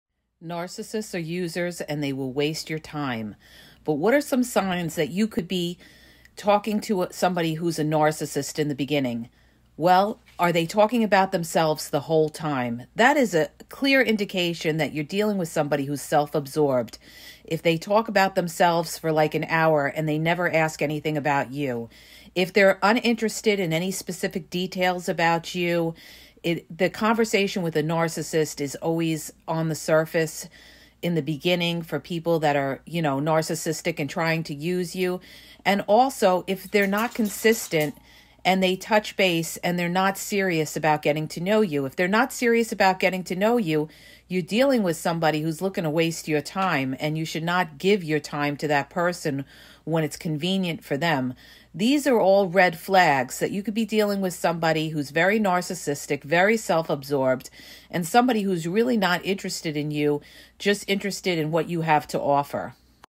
Narcissists are Users Who Waste Your Time
This video talks about how narcissists are users who waste your time.

To ask a question and get a video sent to you go

Read “He’s Not Into You-He’s Wasting Your Time” 👉🏼